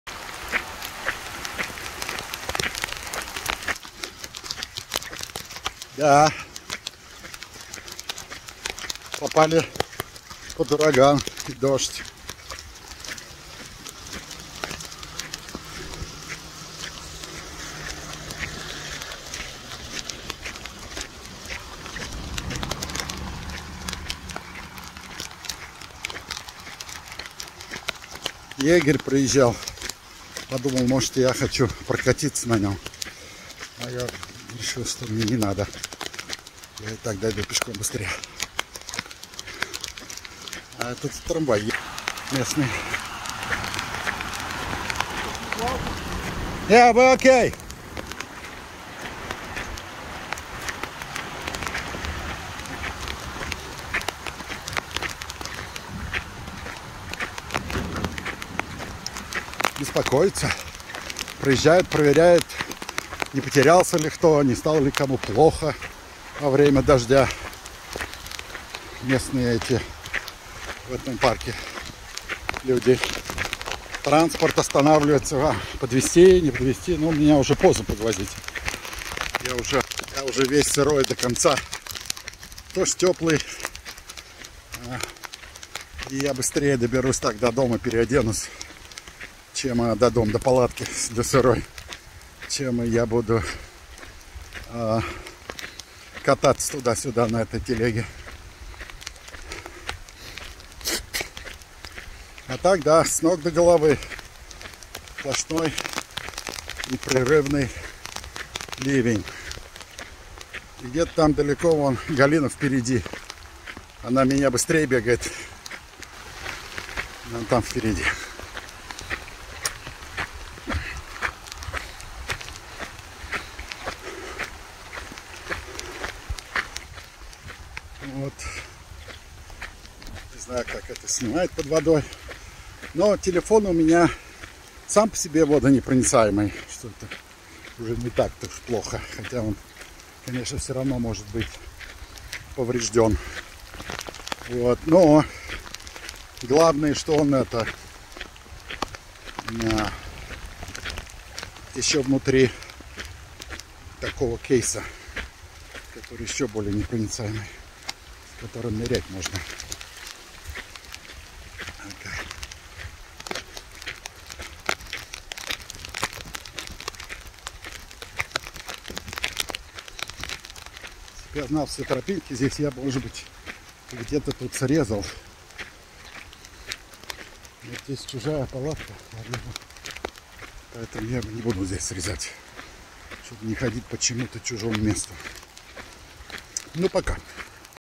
August 29, 2021 Проливной дождь на дороге недалеко от моря
Дождь от урагана Ида. Налетел внезапно.